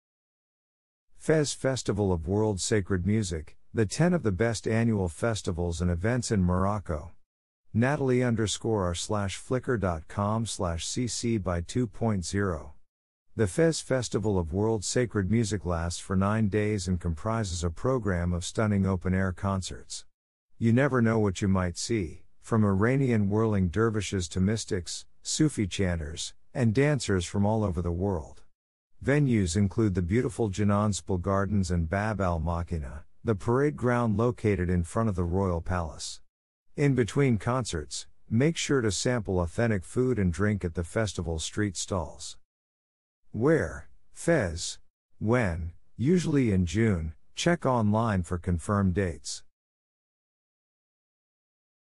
Fez Festival of World Sacred Music
Fez Festival of World Sacred Music - The 10 of the Best Annual Festivals and Events in Morocco.
The Fez Festival of World Sacred Music lasts for nine days and comprises a program of stunning open-air concerts. You never know what you might see, from Iranian whirling dervishes to mystics, Sufi chanters, and dancers from all over the world. Venues include the beautiful Jnan Sbil Gardens and Bab al Makina, the parade ground located in front of the Royal Palace. In between concerts, make sure to sample authentic food and drink at the festival's street stalls. .
Where: Fez.
When: Usually in June, check online for confirmed dates..